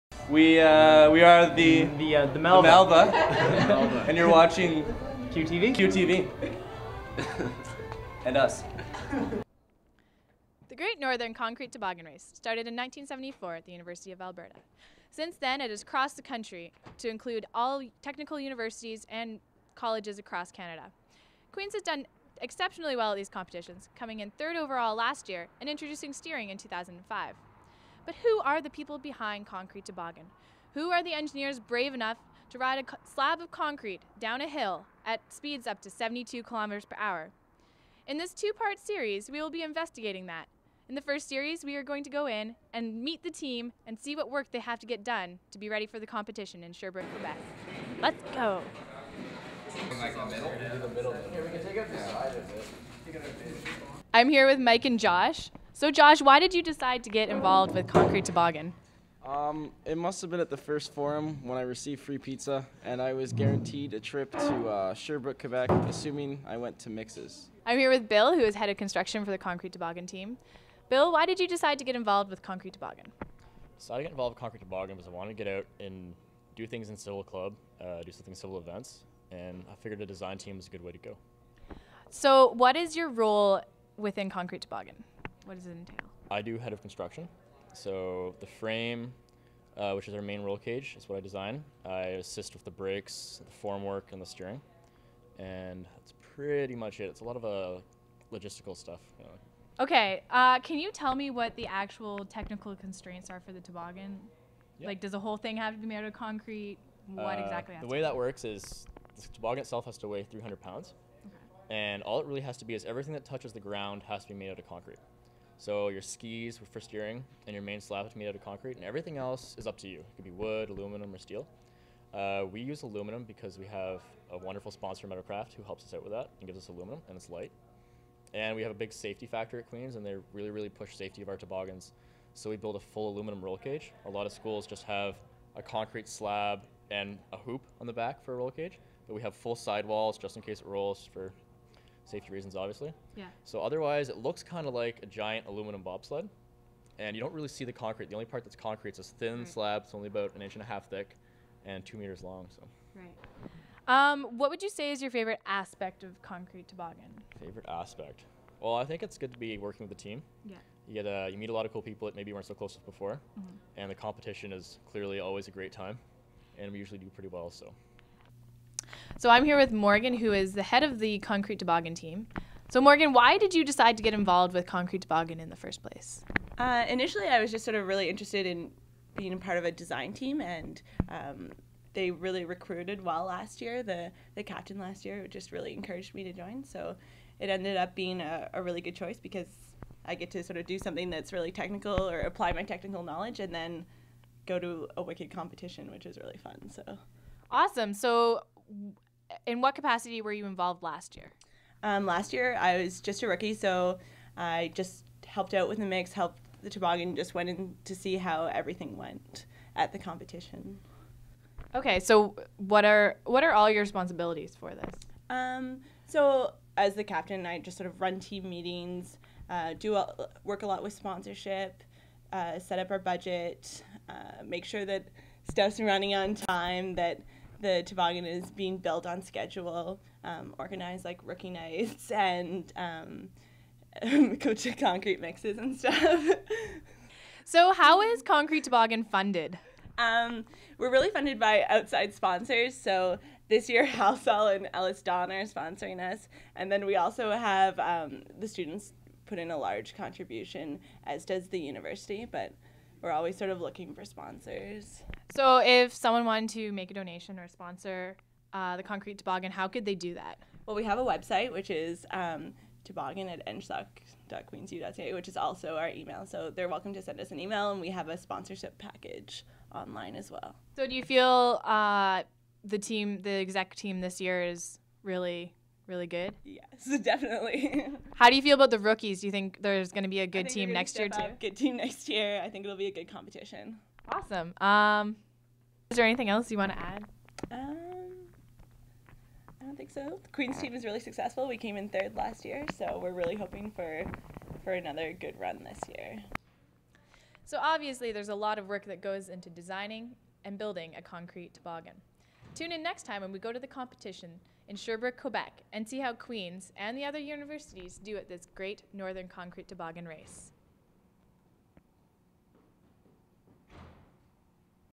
E10 C4 Concrete Toboggan
QTV checks out the concrete toboggan. With Megan Lenz.